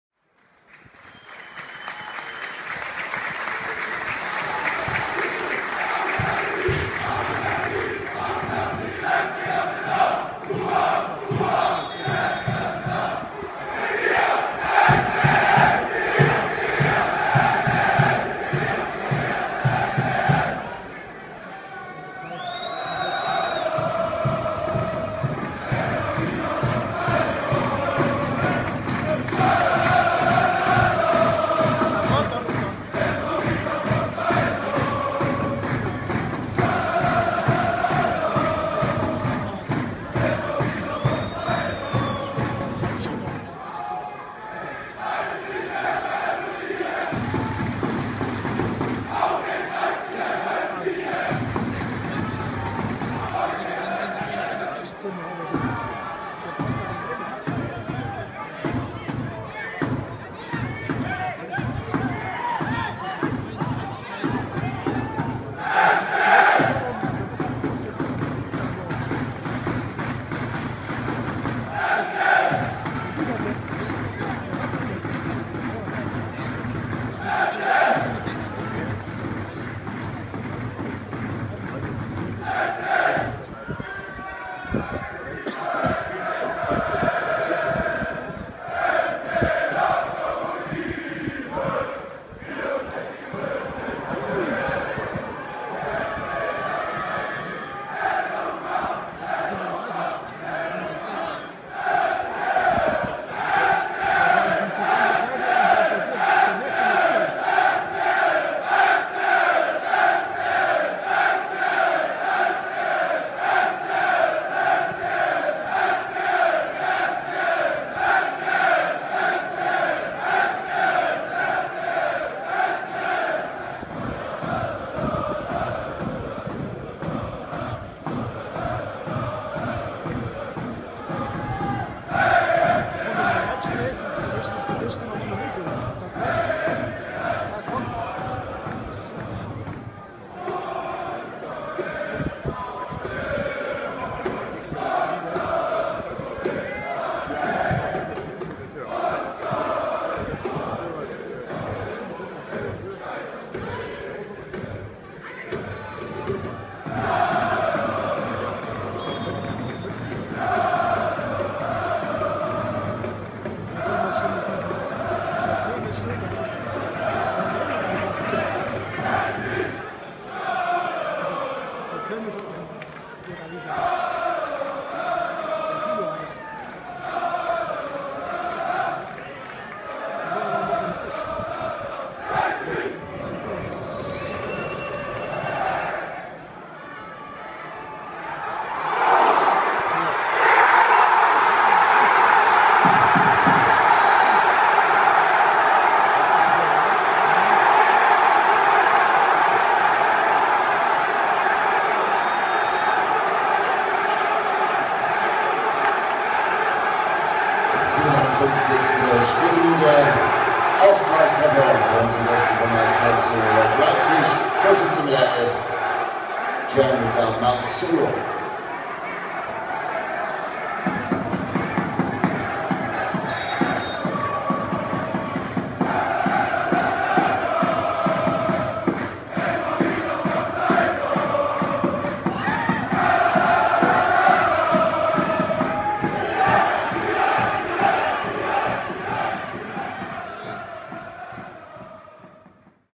Chemnitzer FC II 2:1 1. FC Lokomotive Leipzig 15.02.2015 | Support
NOFV-Oberliga Süd, 10. Spieltag, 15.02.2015
Chemnitzer FC II - 1. FC Lokomotive Leipzig  2:1 (1:0)
Zuschauer: 1.443 (ca. 1.000 Gäste)
Stadion an der Gellertstraße, Chemnitz